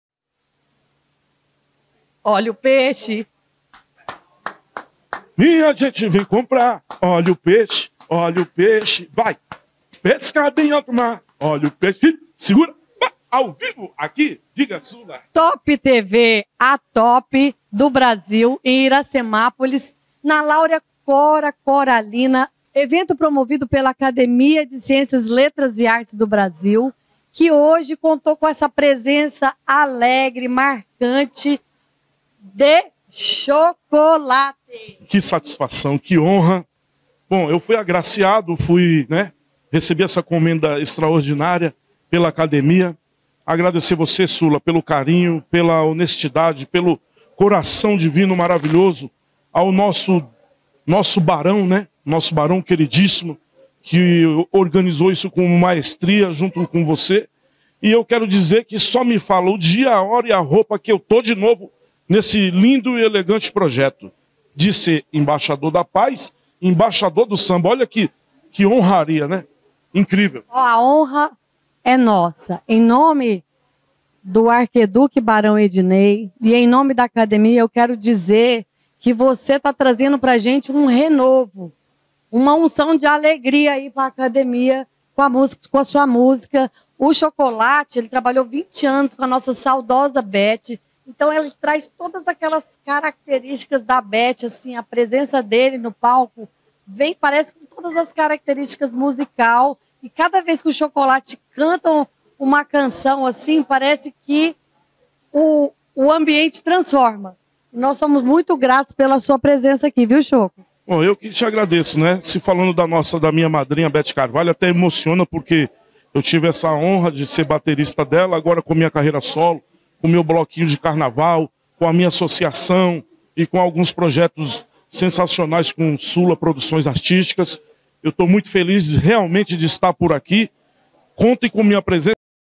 TOP TV por Sula Costa Entrevista Chocolate do Samba | Cerimonia ACLAB
Chocolate do Samba, renomado baterista que por 20 anos acompanhou a saudosa Beth Carvalho nos palcos, foi recentemente homenageado com o título de Embaixador do Samba pela ACLAB -  Academia de Letras e Artes do Brasil. A premiação é um reconhecimento ao talento e dedicação deste músico que tanto contribuiu para a difusão e preservação da cultura do samba.

Em uma emocionante entrevista concedida à jornalista Sula Costa, Chocolate do Samba relembrou os momentos marcantes de sua carreira ao lado de Beth Carvalho, destacando a importância da cantora para o samba brasileiro. Com sua batida contagiante e seu jeito único de interpretar os ritmos do samba, o músico se tornou um ícone para gerações de amantes da música popular brasileira.

Além de sua atuação brilhante nos palcos, Chocolate do Samba também se dedica a projetos sociais que promovem a inclusão e valorização da cultura do samba nas comunidades mais carentes. Sua paixão pela música e seu compromisso com a transformação social são admiráveis e fazem dele um verdadeiro embaixador do samba.

Com sua simpatia e humildade, Chocolate do Samba conquistou o coração de milhares de fãs ao redor do mundo e deixou um legado inestimável para a música brasileira. Que sua trajetória sirva de inspiração para as futuras gerações de músicos e que seu talento continue a ecoar por muitos e muitos anos. Parabéns, Embaixador do Samba!
Transcript